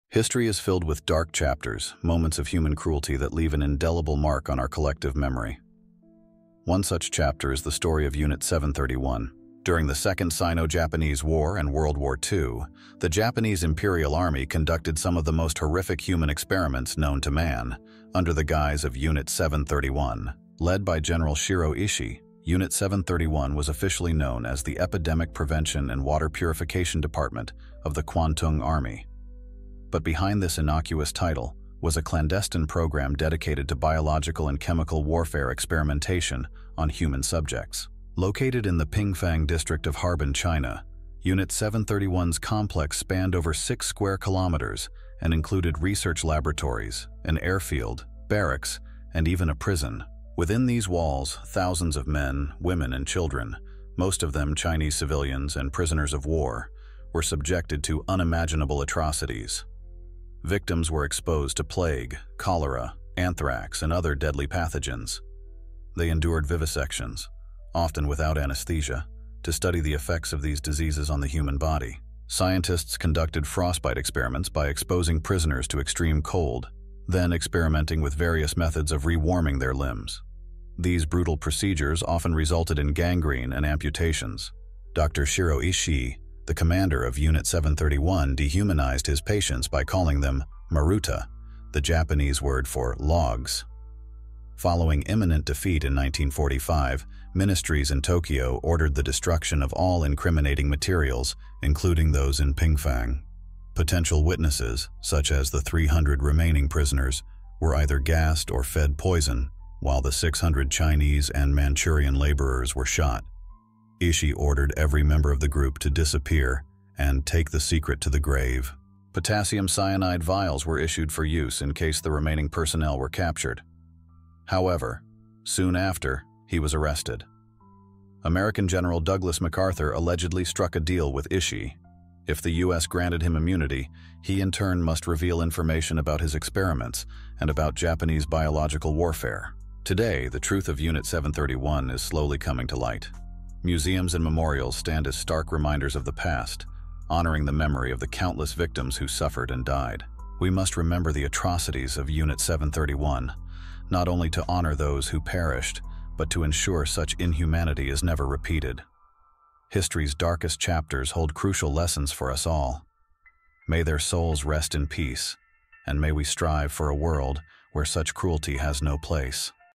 Unit 731 - What is it? Uncovering Japan's WWII War Crimes
In this chilling documentary, we delve into the dark history of Unit 731, a covert biological and chemical warfare research and development unit of the Imperial Japanese Army during World War II. Led by General Shiro Ishii, Unit 731 conducted inhumane experiments on thousands of innocent civilians and prisoners of war, leaving a legacy of suffering and death.

Join us as we uncover the truth behind the horrific experiments, the victims' harrowing experiences, and the aftermath that allowed many perpetrators to evade justice. Through archival footage, historical documents, and survivor testimonies, we remember the victims and learn the crucial lessons this dark chapter of history holds for us all.